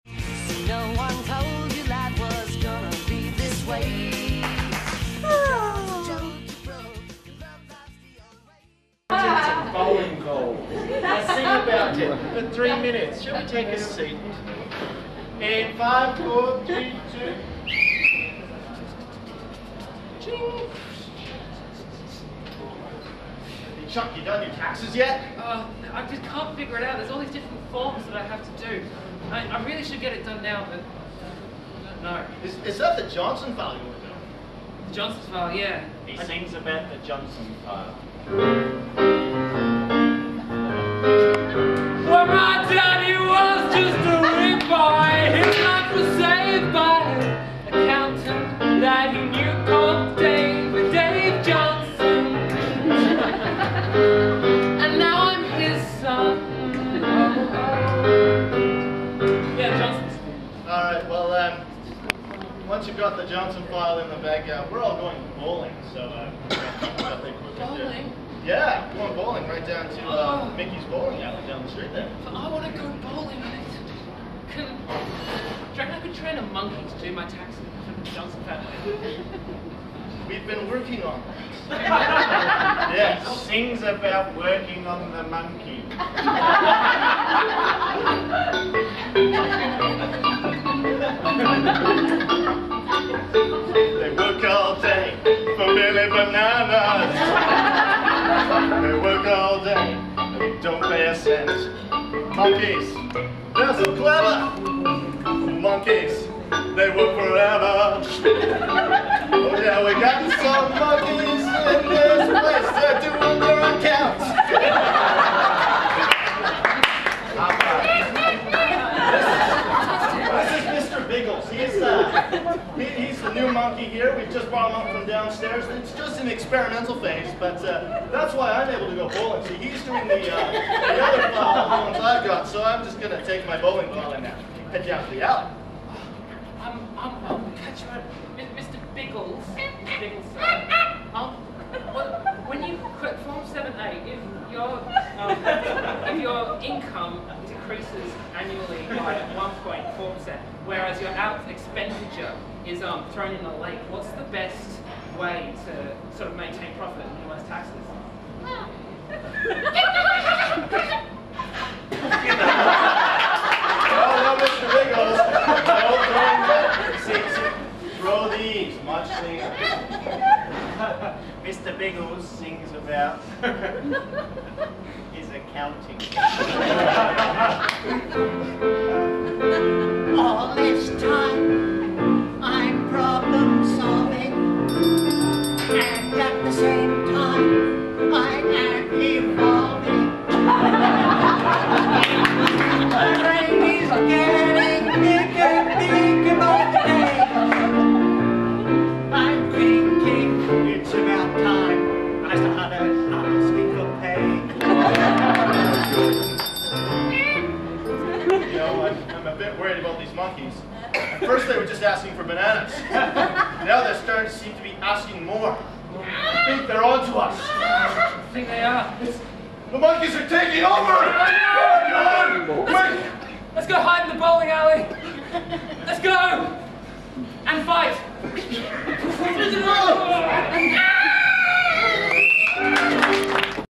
Scriptless 27-11-08 'Sing About It'
Thursday, November 27 - Round 4 of the Highlander teams competition at Scared Scriptless. The 4 Musketeers perform a 'Sing About It' scene with the offer of 'a bowling ball'.

The performers are Ryan Blouin, Rachel Bond, Steve Johnston & Jim Fishwick.